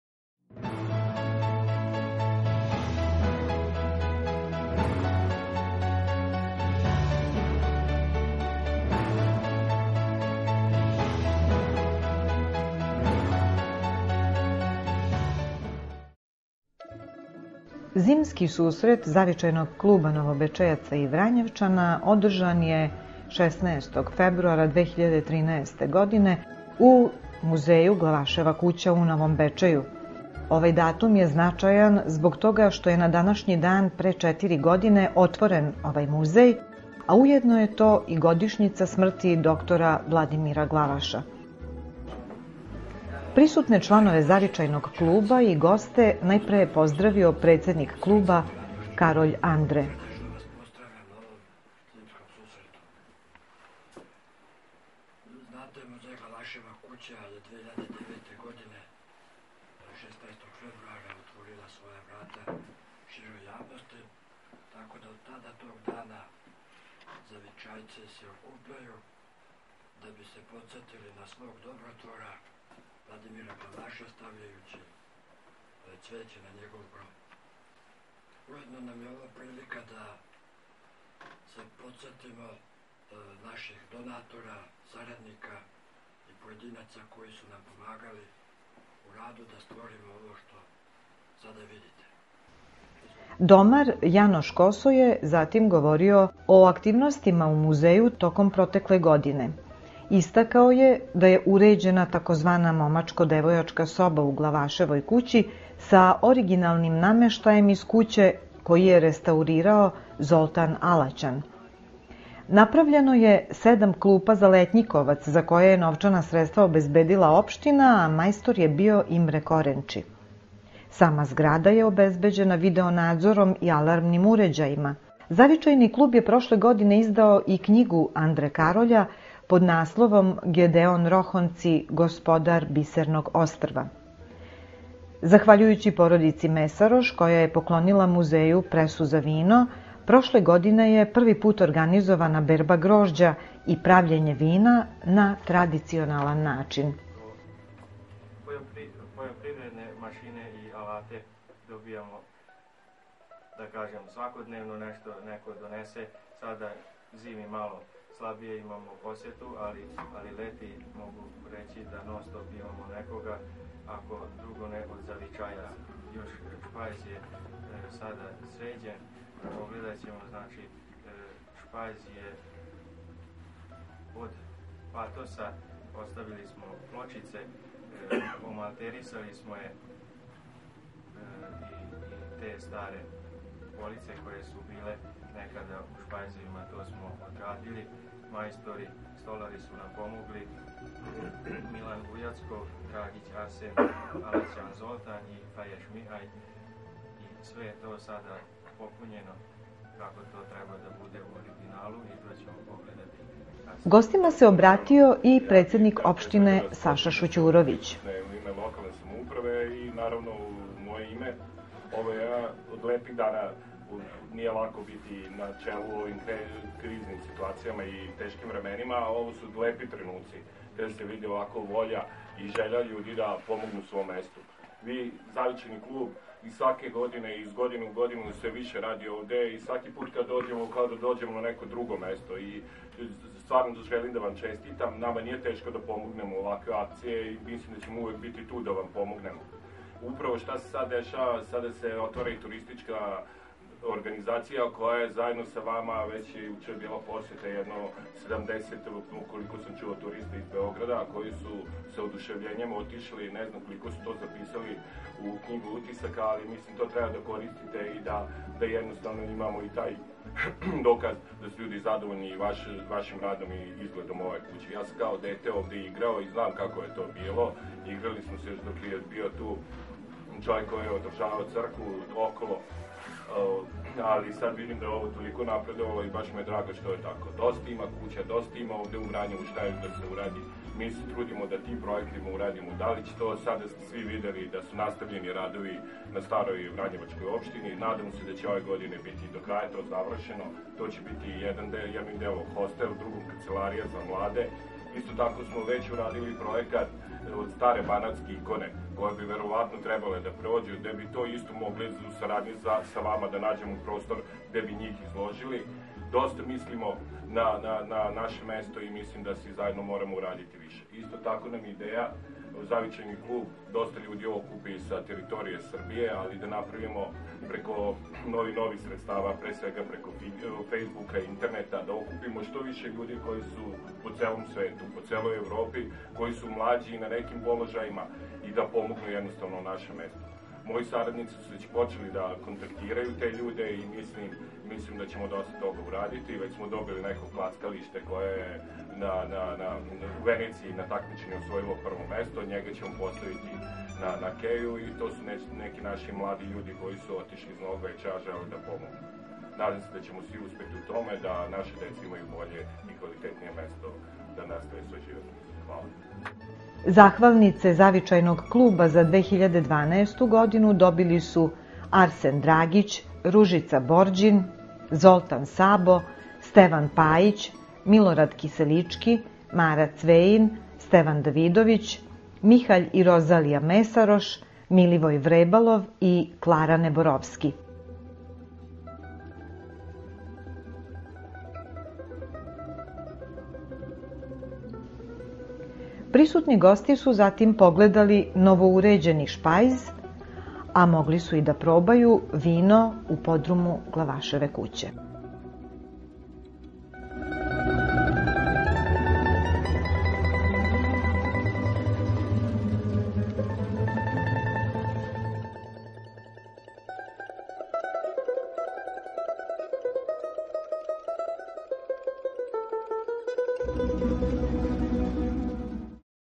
Zimski susret zavičajaca
Zimski susret zavičaaca održan je u Novom Bečeju, 16. februara 2013.godine u Muzeju "Glavaševa kuća"